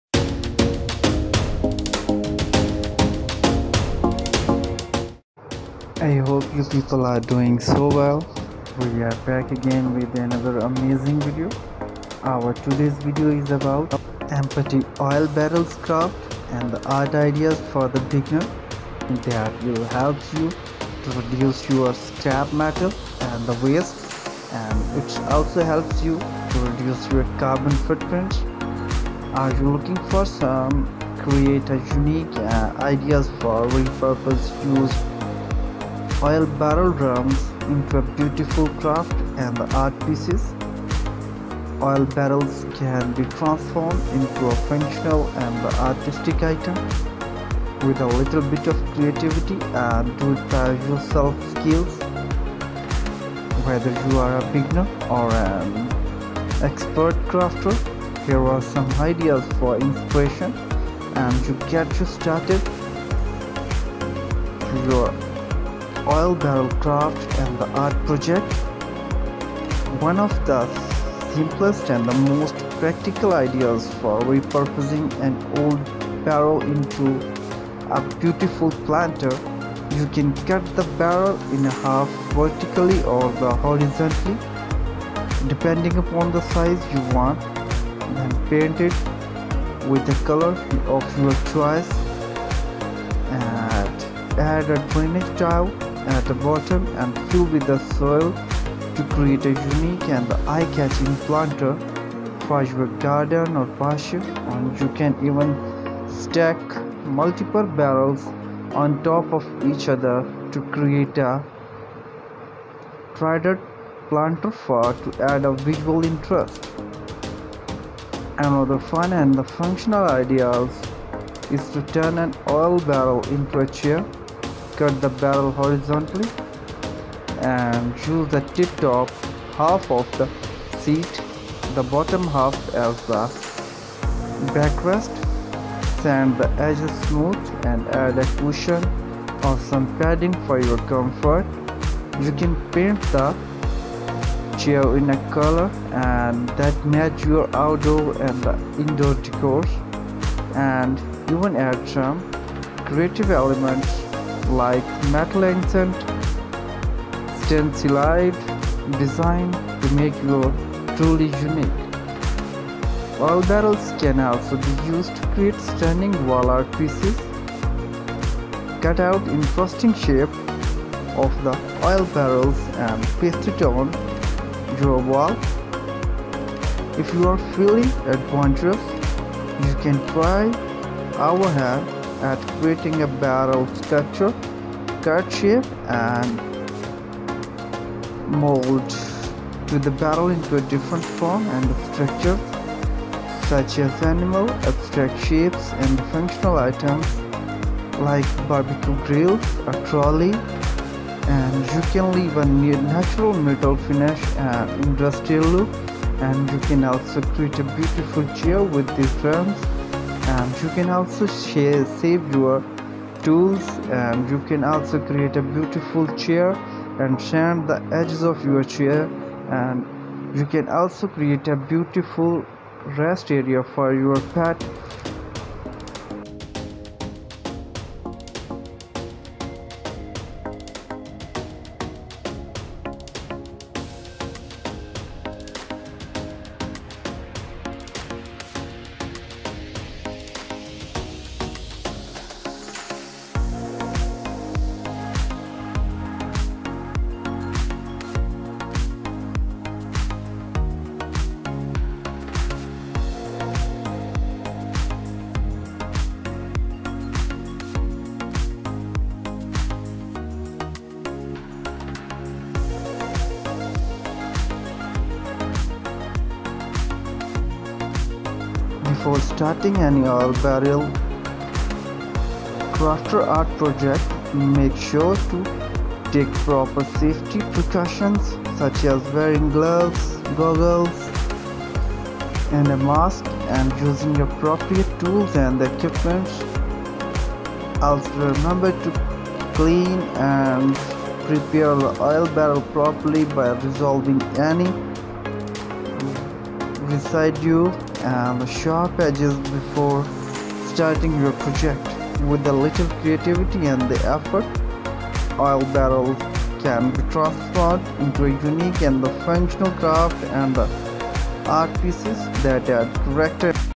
50+ unique DIY ideas from Metal Gallon Oil Drums/barrels | upcycle | metal furniture | art
50+ Unique DIY Ideas from Metal Gallon Oil Drums/Barrels | Upcycle | Metal Furniture | Art"

Discover over 50 creative and innovative DIY ideas for upcycling metal gallon oil drums/barrels into stunning furniture and art pieces. From unique home decor to functional furniture, this video will inspire you to transform discarded metal barrels into beautiful and functional pieces. Whether you're a DIY enthusiast, a metalworking artist, or just looking for eco-friendly ways to repurpose materials, this video has something for everyone. Get ready to be amazed by the endless possibilities of upcycling metal barrels and unlock your creativity with these unique DIY ideas!